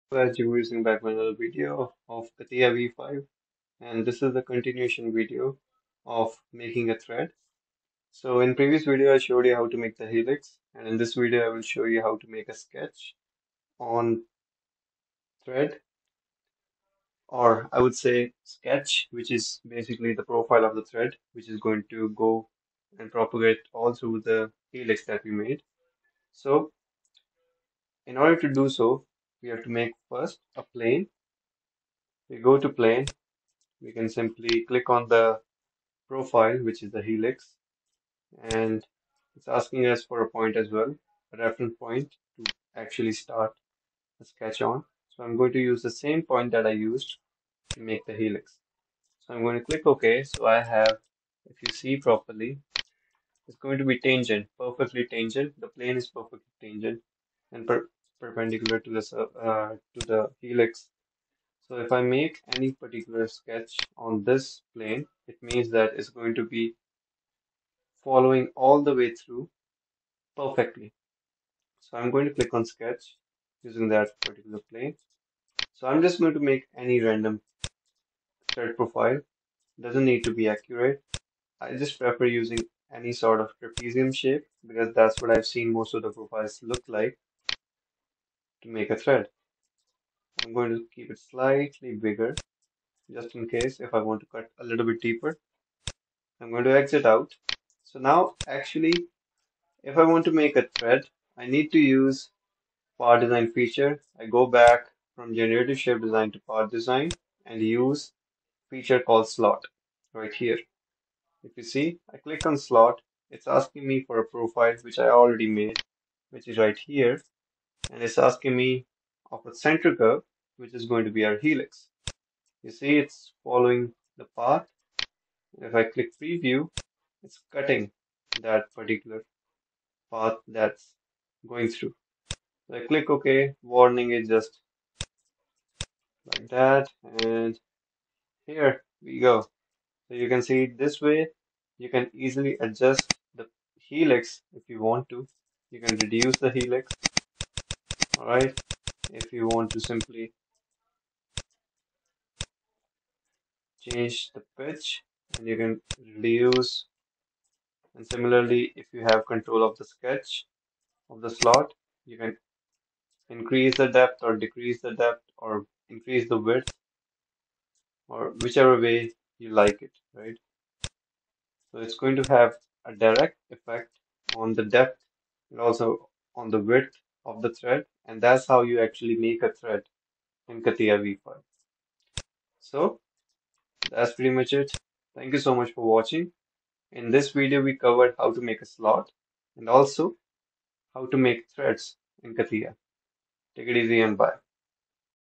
CATIA V5 : HOW TO USE SLOT FEATURE ?
Learn how to use power slot feature to make beautiful profiles.
Stayed tuned to learn more and ask questions in comments